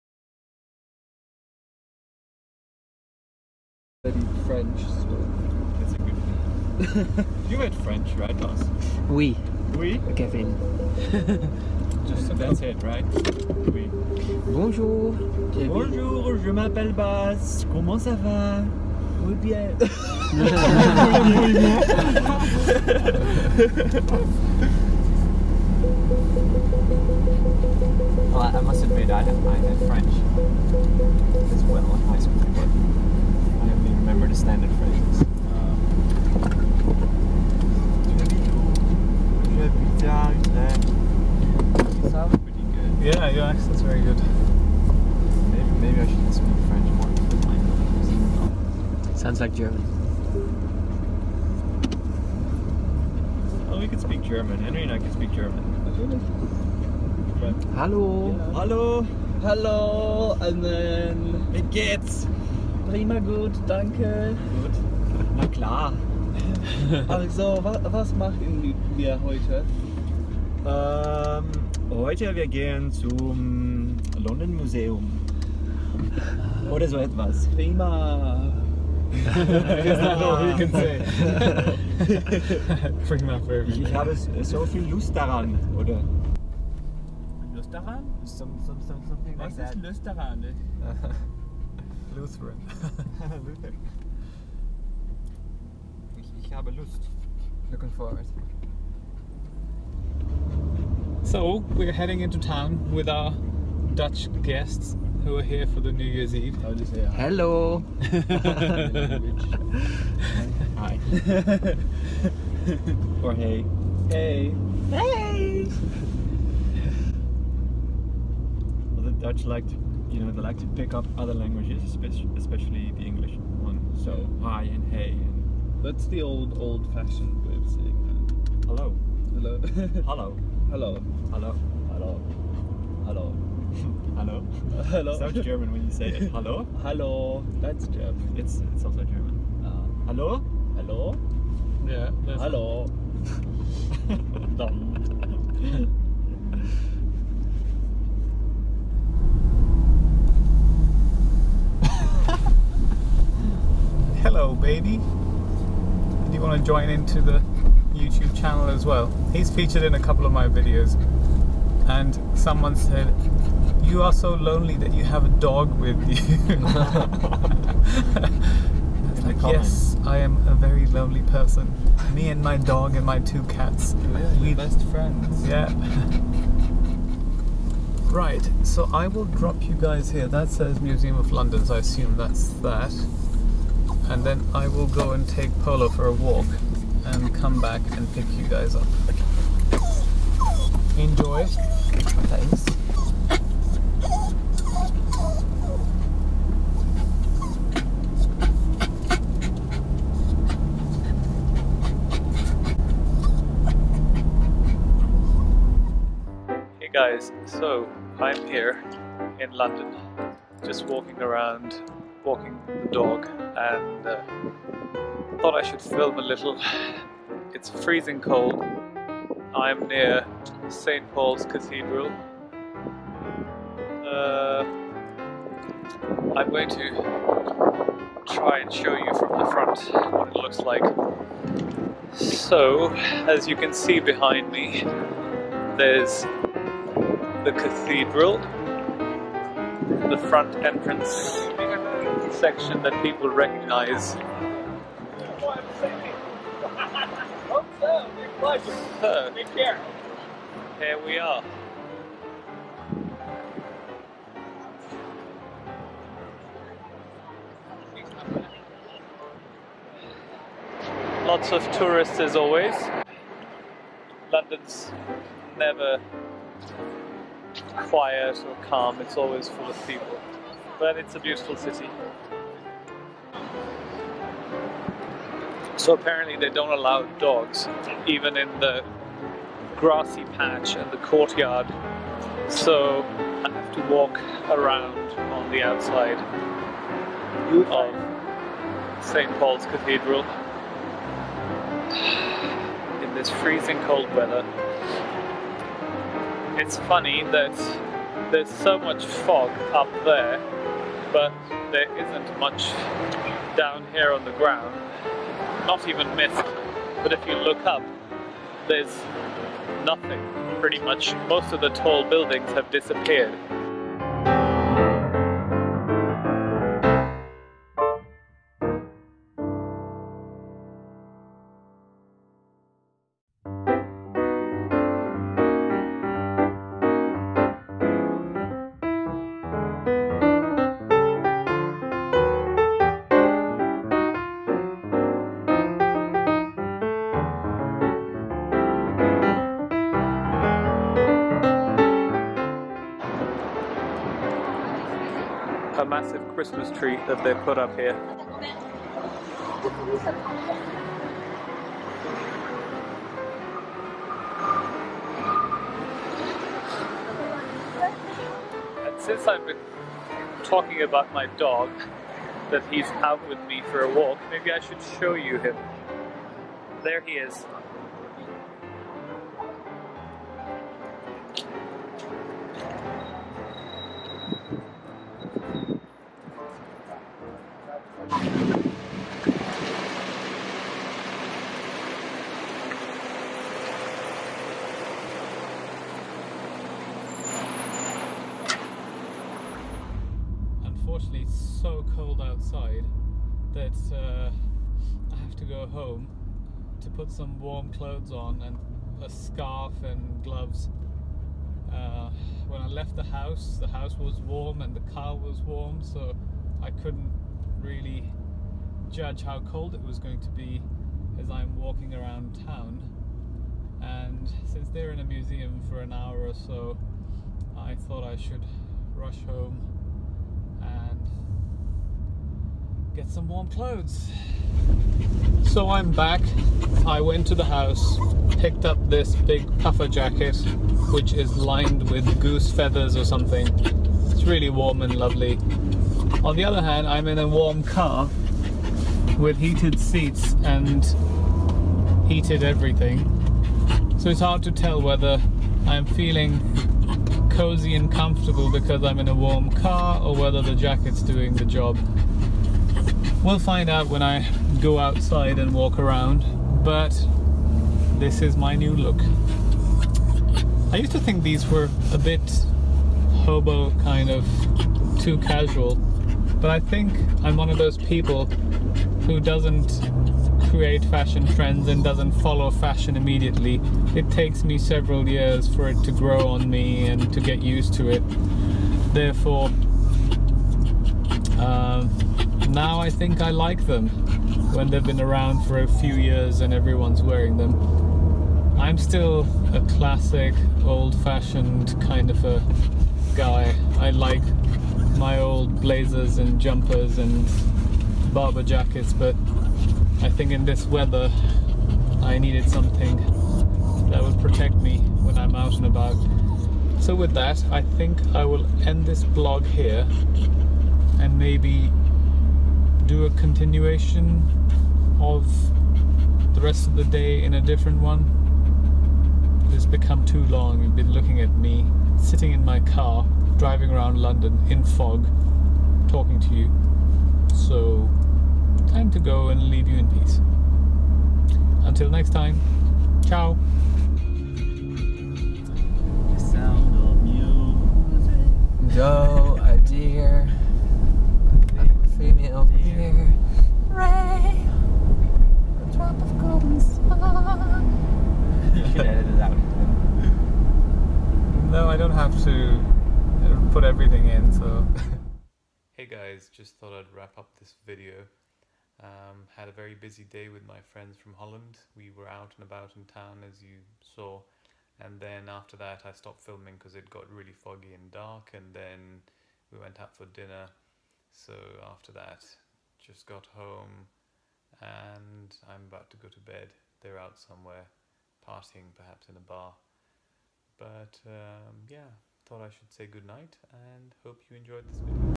Dutch Visitors in London - VLOG | 022
Out and about in London with my friends in fog and freezing cold but the Panamera kept us warm and comfortable.